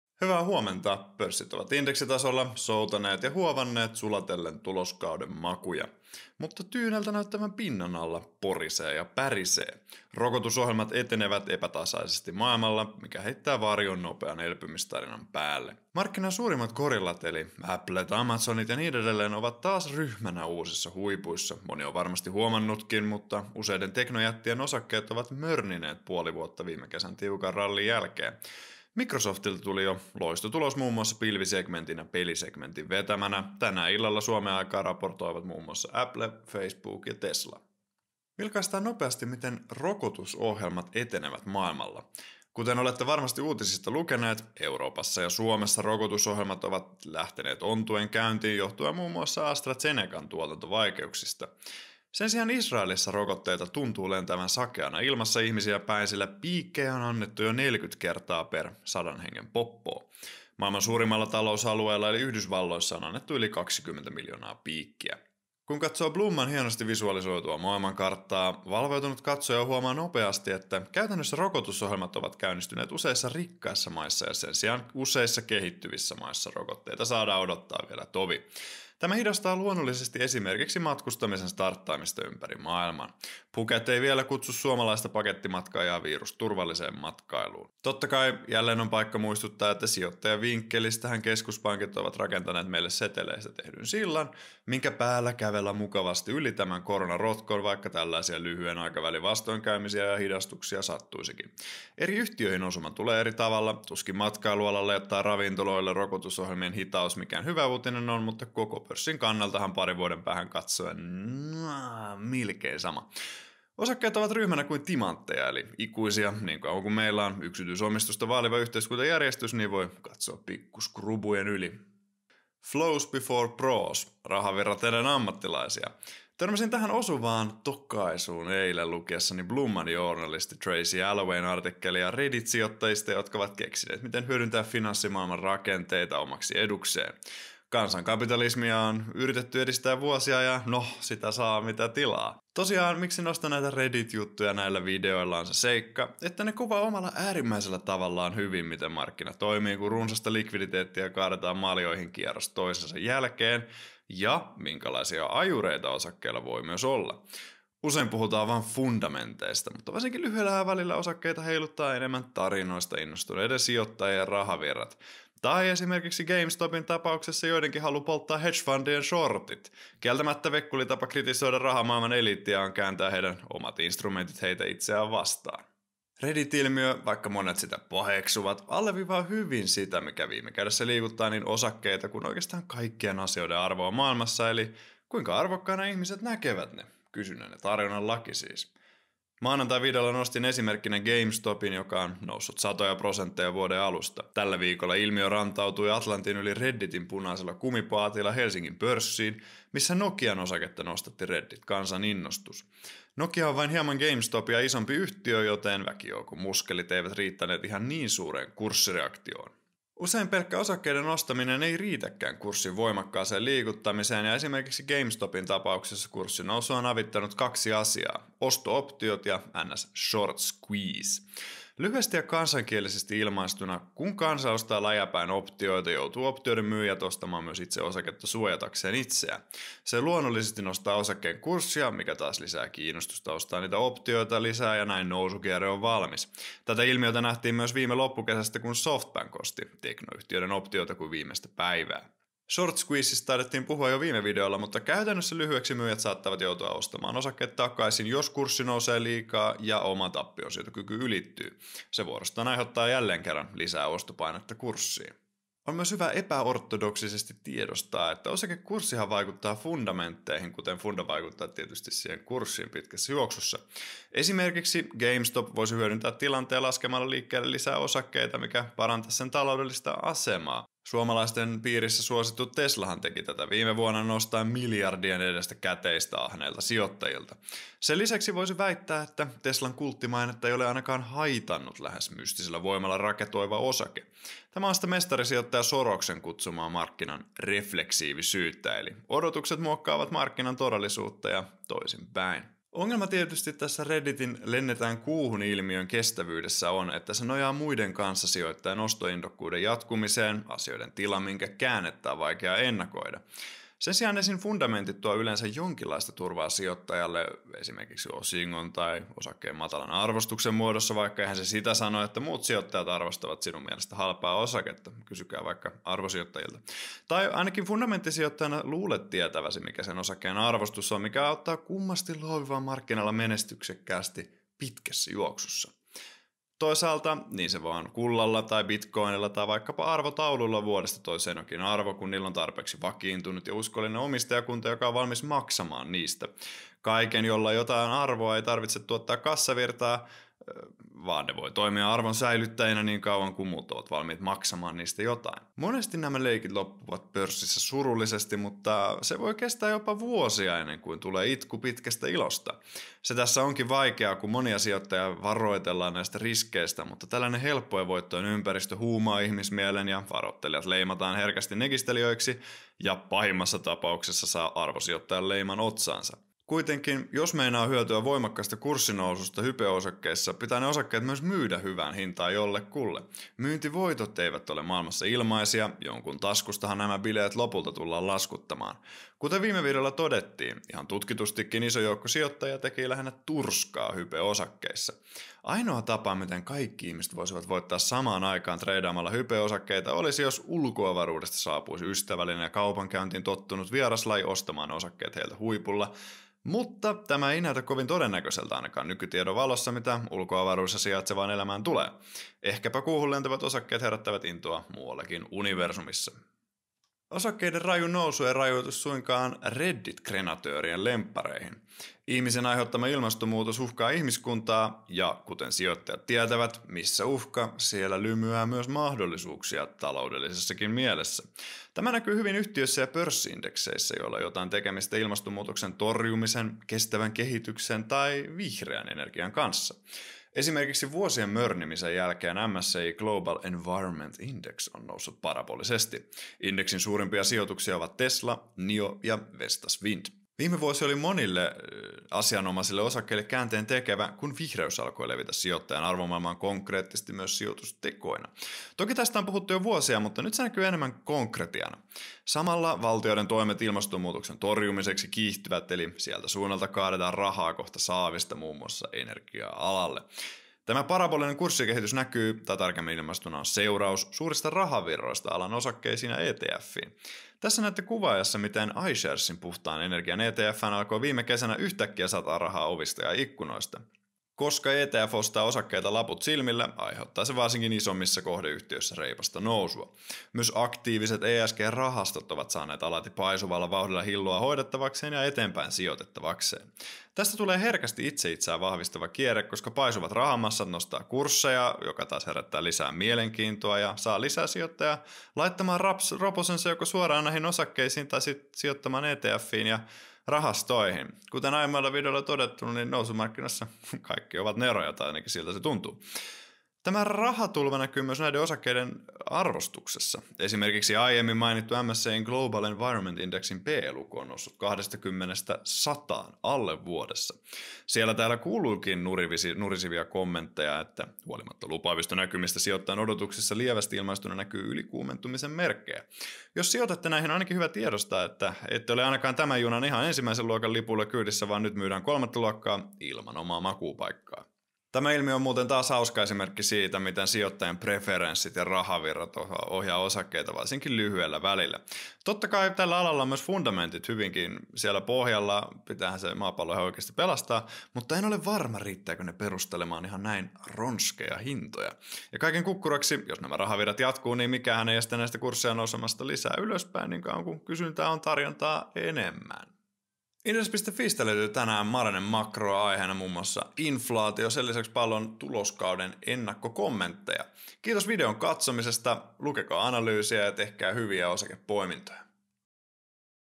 Lennetään kuuhun!
Lennetään kuuhun! – Flows before Pros – Kuplanpoikasta kestävässä kehityksessä? – Virus-vapaa pakettimatka Phuketiin – Näin se pörssi oikeasti toimii – Kamalin leima, minkä sijoittaja voi saada